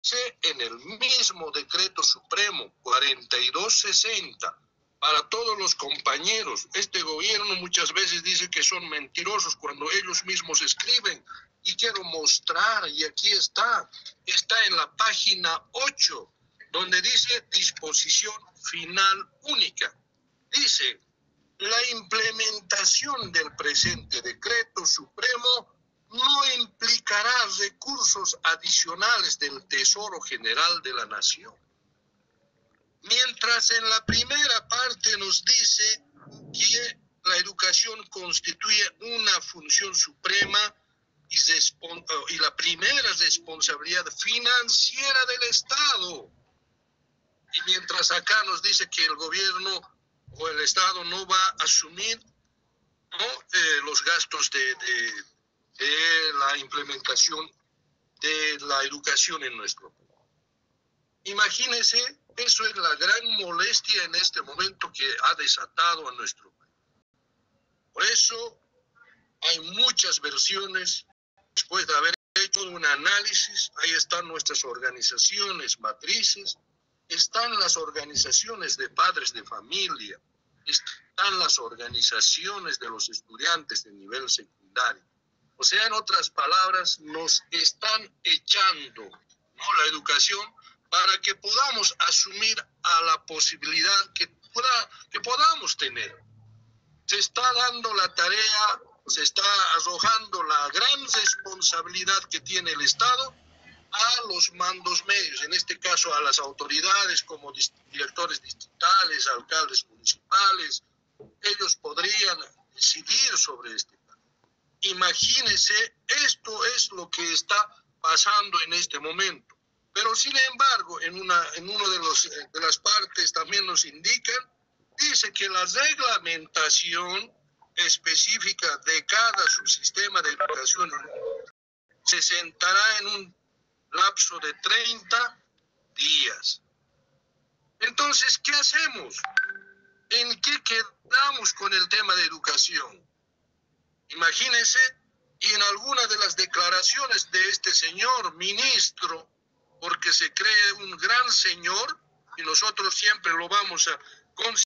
*RESUMEN DE LA JORNADA VIRTUAL* *16/06/2020* *"CRISIS EDUCATIVA EN BOLIVIA"* POR NUESTRO COMPAÑERO Y
PROF. FREDDY MAMANI LAURA EX EJECUTIVO NACIONAL DE LA CONMERB